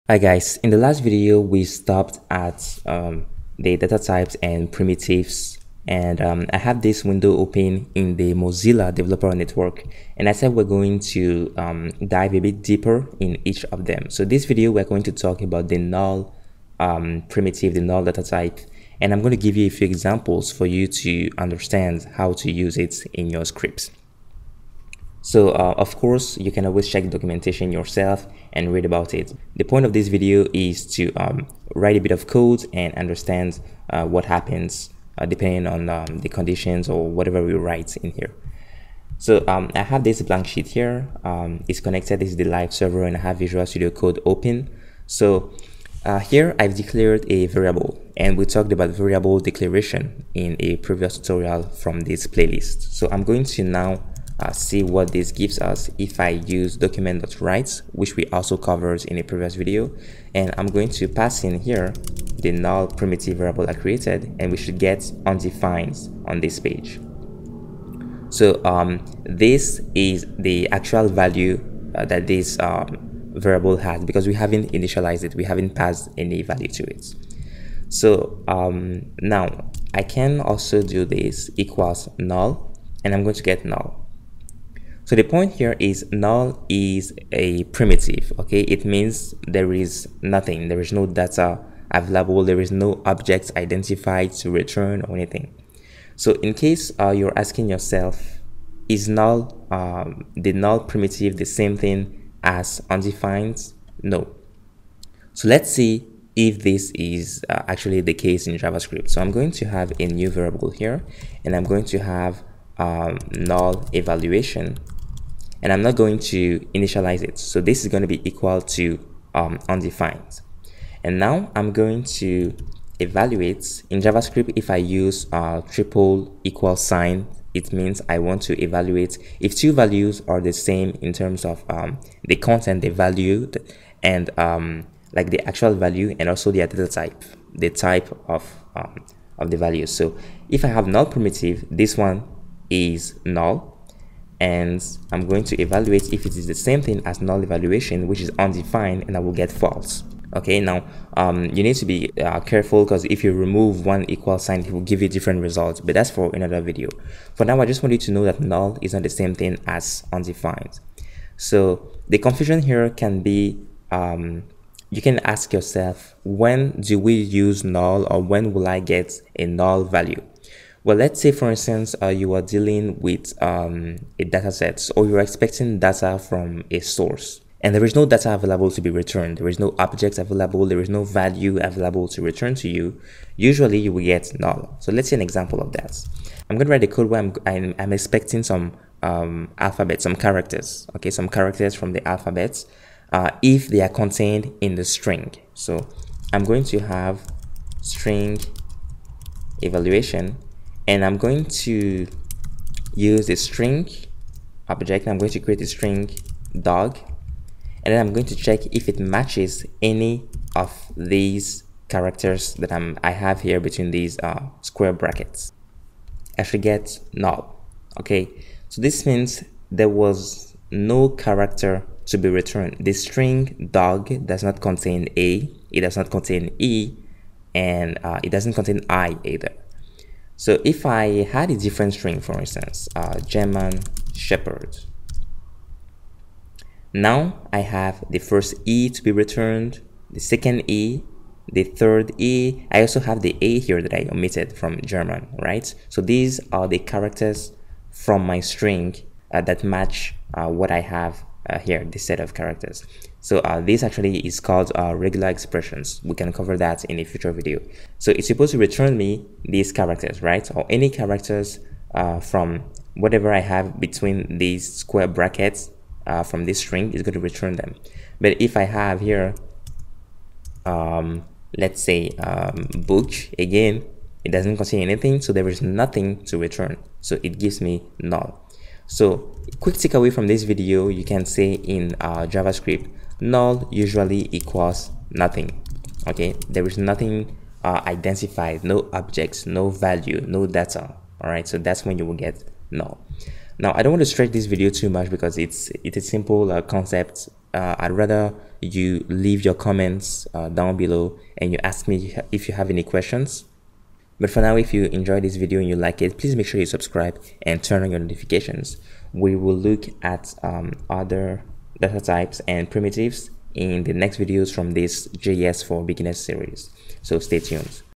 Javascript null data type explained | JS for Beginners Ep. 14
The null data type in JavaScript is a primitive value that is used to indicate the absence of an object or data. In this beginner tutorial, we look at examples showcasing the difference between null and undefined, and also when we should expect to get a null value returned to us.